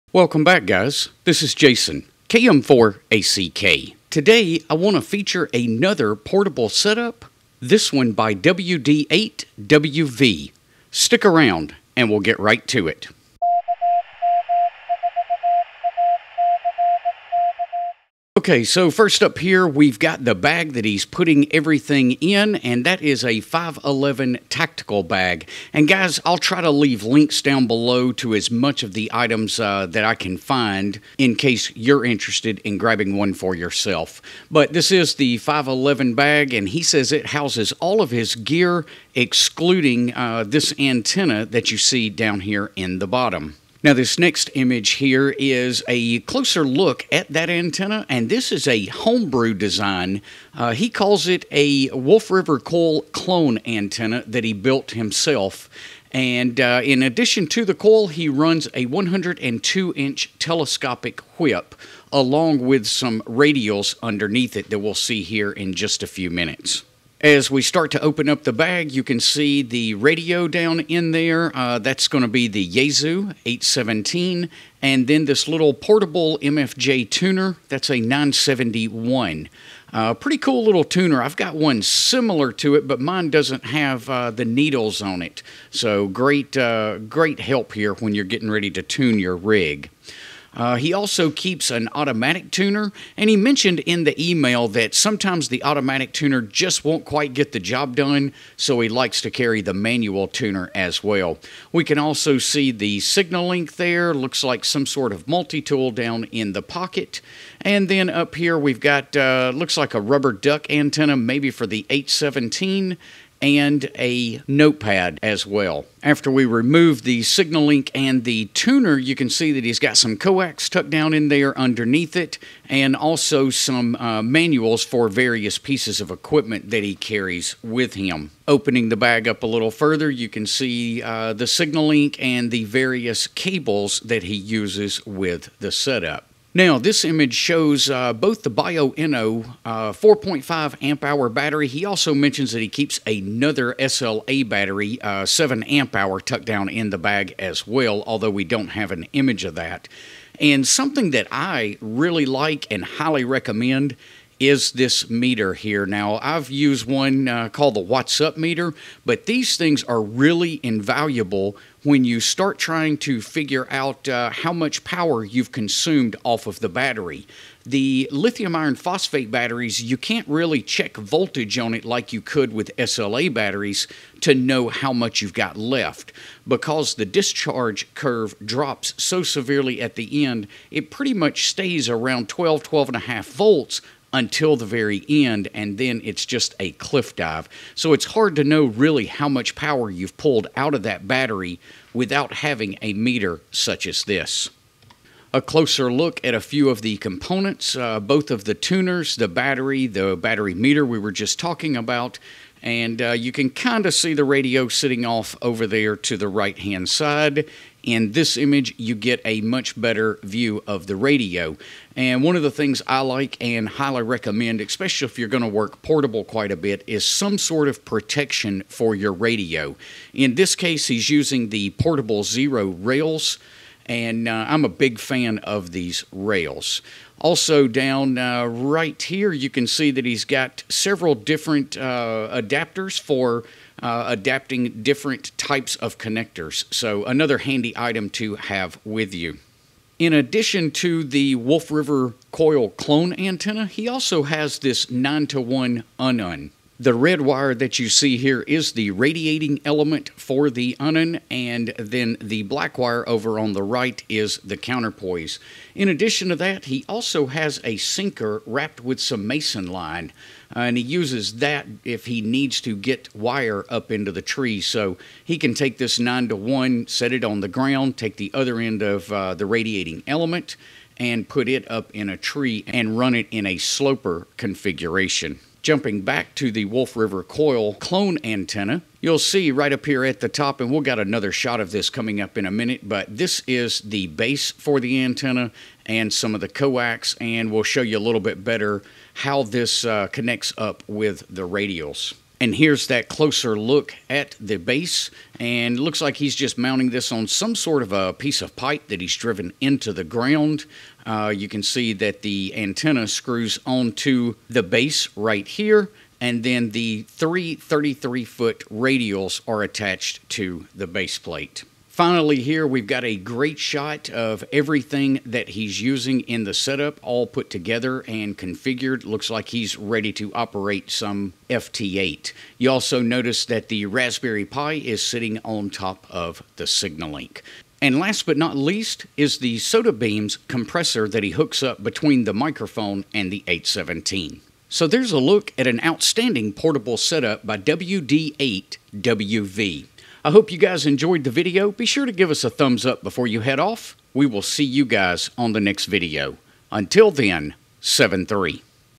Portable Showcase WD8WV
5.11 Backpack

Watts Up Meter

Bioenna Battery

Signalink Sound Card

Auto Tuner

Yaesu 817ND